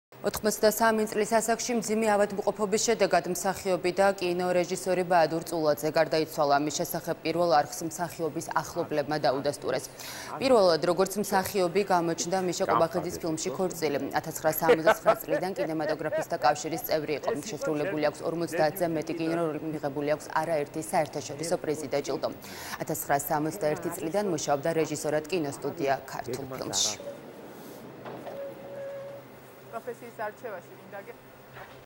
მსახიობი და კინორეჟისორი ბაადურ წულაძე გარდაიცვალა
83 წლის ასაკში მძიმე ავადმყოფობის შედეგად მსახიობი და კინორეჟისორი ბაადურ წულაძე გარდაიცვალა. ამის შესახებ პირველ არხს მსახიობის ახლობლებმა დაუდასტურეს.
პირველად როგორც მსახიობი გამოჩნდა მიშა კობახიძის ფილმში „ქორწილი”. 1969 წლიდან კინემატოგრაფისტთა კავშირის წევრი იყო, შესრულებული აქვს ორმოცდაათზე მეტი კინოროლი. მიღებული აქვს არა ერთი საერთაშორისო პრიზი და ჯილდო. 1961 წლიდან მუშაობს რეჟისორად კინოსტუდია ქართულ ფილმში.